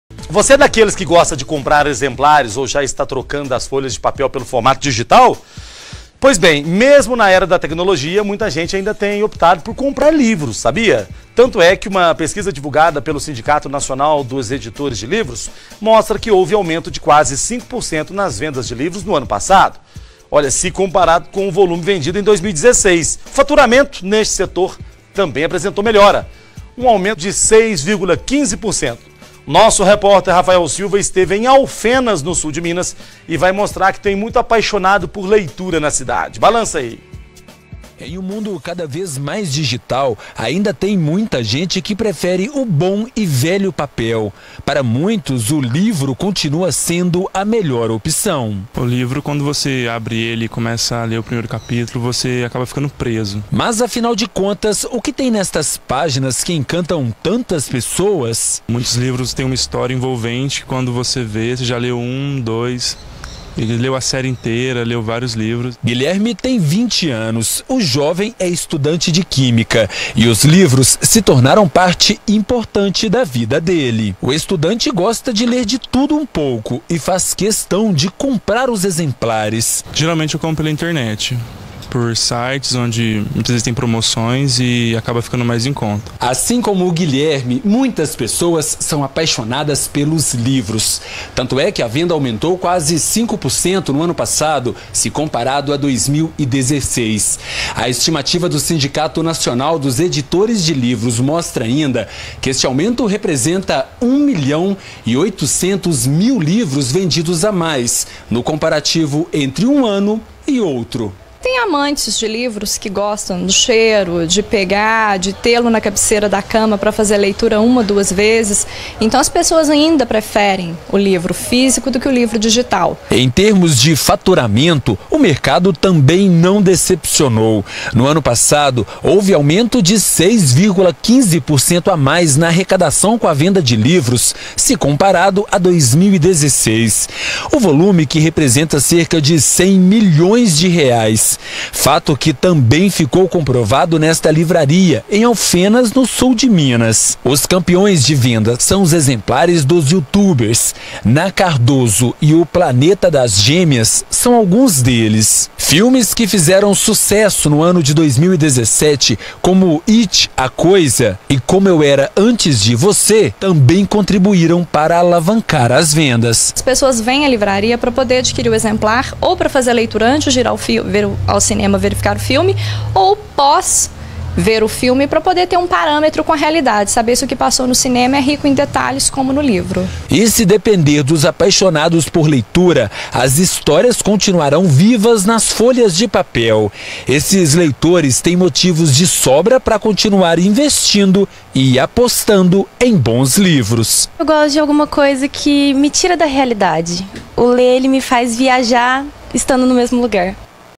Mesmo em tempos digitais: venda de livros aumenta no Brasil
Você é daqueles que gosta de comprar exemplares ou já está trocando as folhas de papel pelo formato digital? Pois bem, mesmo na era da tecnologia, muita gente ainda tem optado por comprar livros. Tanto é que uma pesquisa divulgada pelo Sindicato Nacional dos Editores de Livros, mostra que houve aumento de quase 5% nas vendas de livros no ano passado, se comparado com o volume vendido em 2016. O faturamento nesse setor também apresentou melhora. Um aumento de 6,15%. Acompanhe com o repórter Rafael Silva.